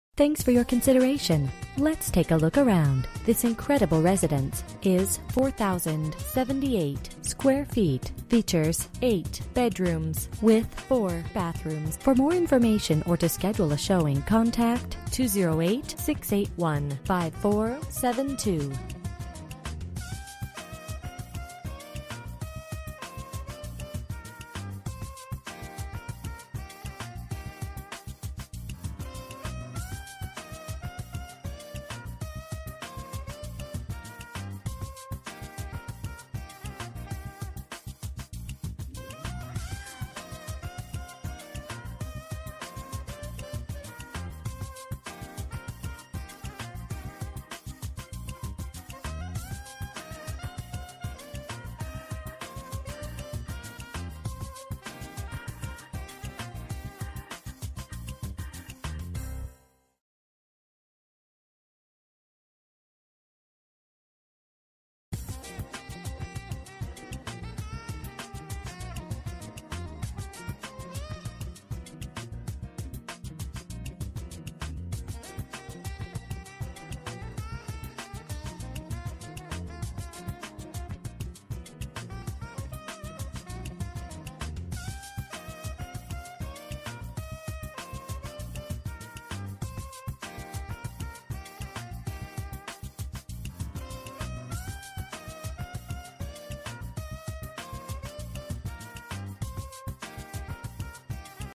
Homes for sale - 4062 N Olsen Street, IONA, ID 83427
This amazing home sits in a rural setting that gives a country feel without being very far from anything you may need. It features 8 bedrooms and 4 bathrooms, so lots of space for everyone. The home was remodeled in 2007 and added the mother-in-law suite and duct work for gas forced-air and air conditioning. The main floor has a mother-in-law suite with kitchen, bathroom and its own laundry closet. There are two fireplaces in the home to lounge near on those cold winter nights. Out back you will find a beautifully landscaped yard with a large playhouse. There is also a large 60 x 50 shop/garage that is perfect for working on all your projects. This beautiful home is waiting for you to come see it today!

Bedrooms: 8
Bathrooms: 4 Bathrooms
Square Feet: 4078
Price: $295,000

10/29/2016 12:37:03 am

Last modified: